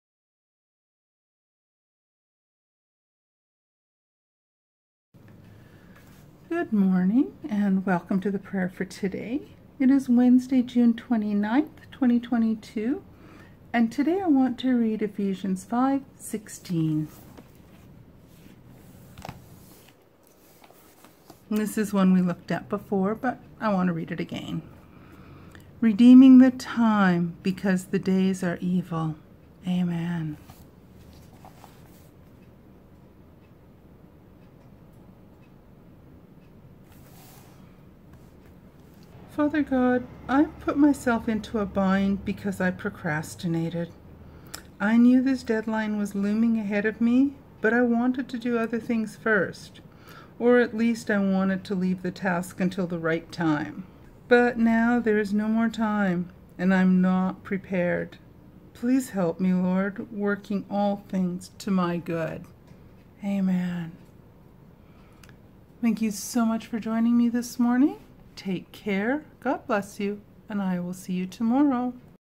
Prayer of the Day - June 29th 2022
Welcome to my little prayer for the day for 2022!  I plan to read a short scripture and have a short prayer each and every day throughout 2022.  I hope you will join me ♥

♥ Karen